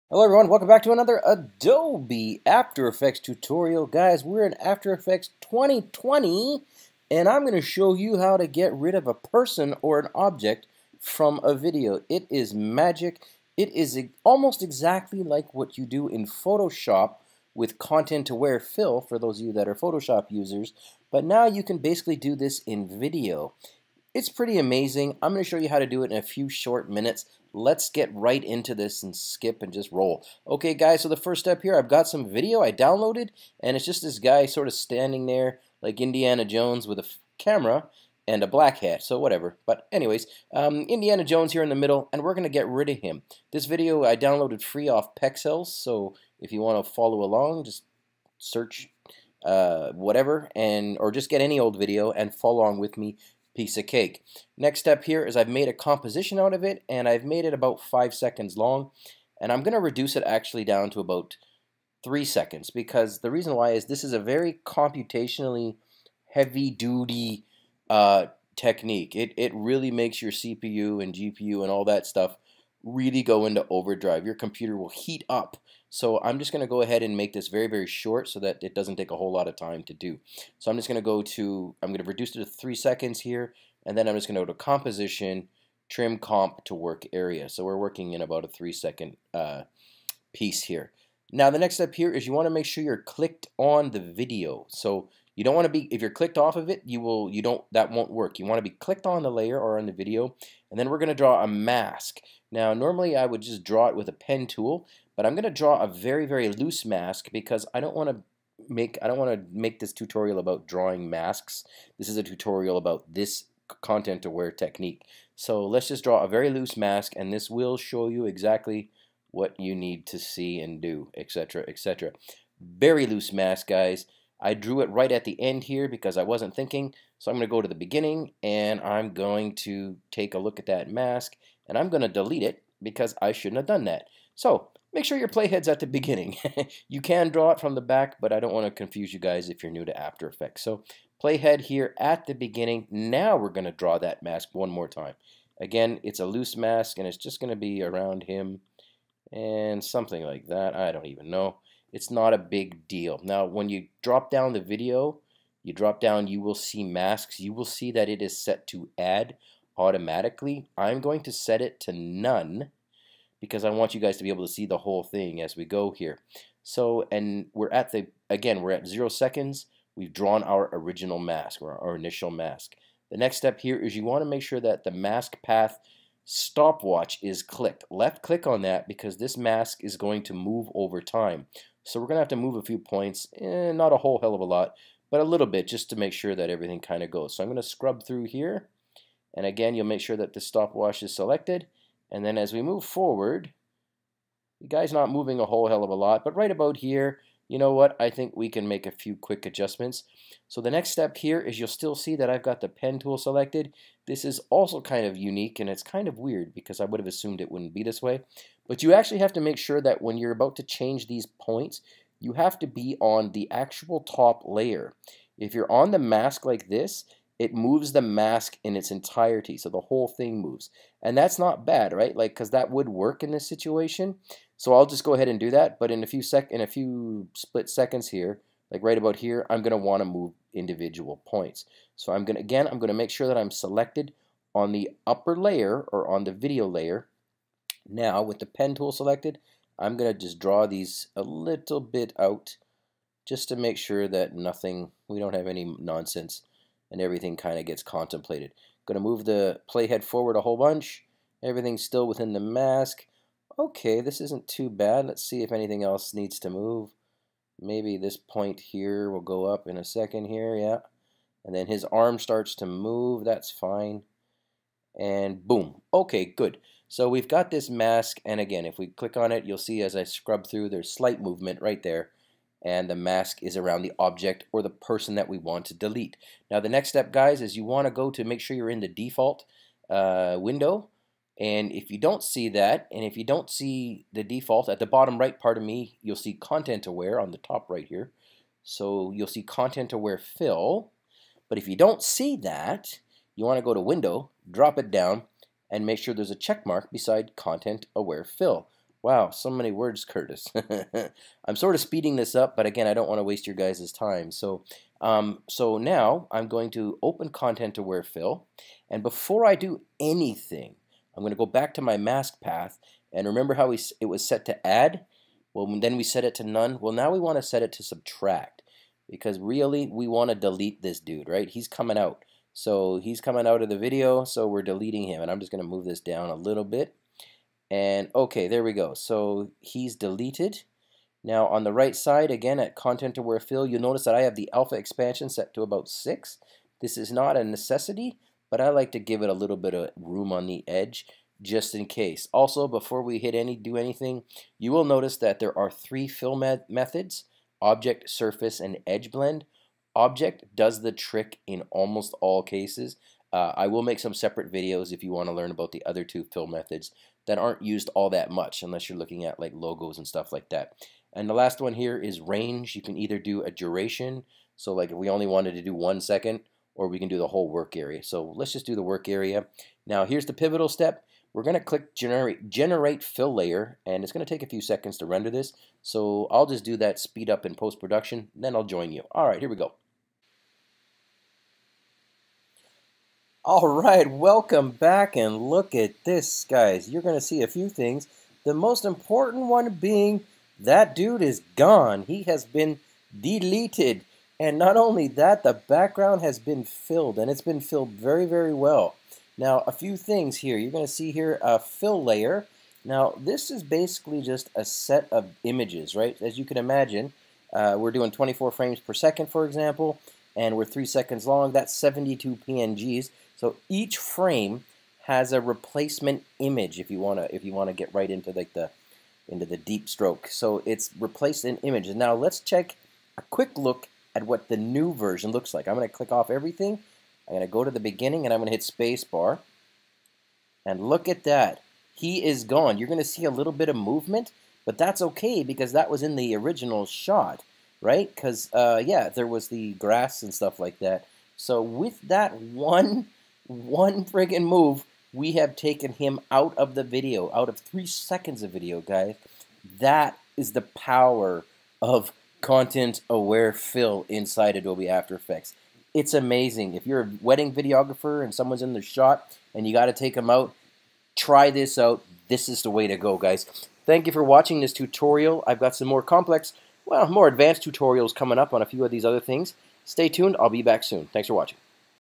How to remove someone from a video - Content Aware Fill - After Effects 2020
In this video, I show you how to remove a person or an object from a video using After Effects 2020 and content aware fill.  This is a tricky process, and it involves a bunch of steps.